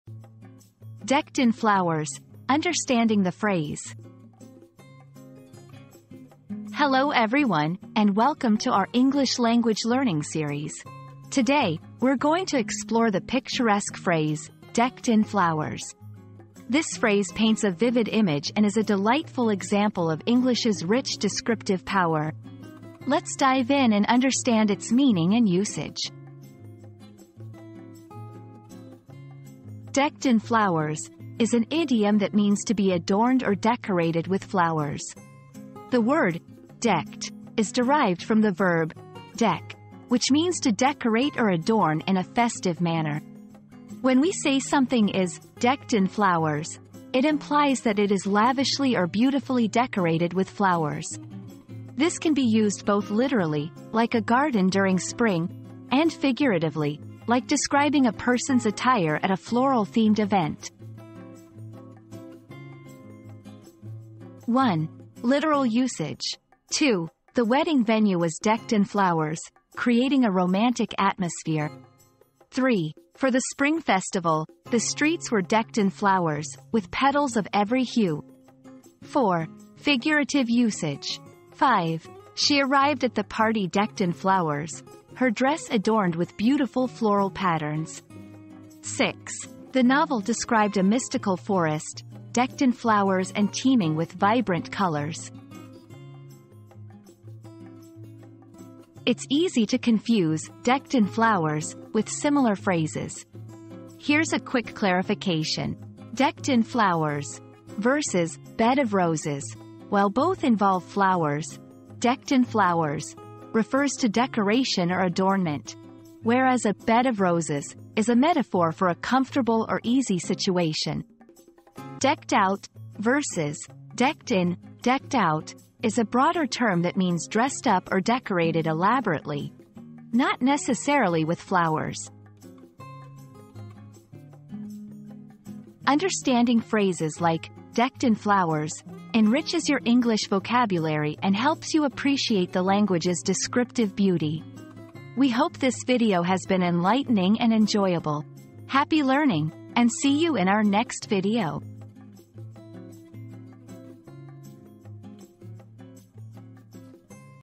Decked in Flowers: Understanding the Phrase
00:00 • Introduction - Decked in Flowers: Understanding the Phrase
00:29 • The Meaning of "Decked in Flowers"
01:03 • Examples of Usage
01:38 • Common Confusions and Clarifications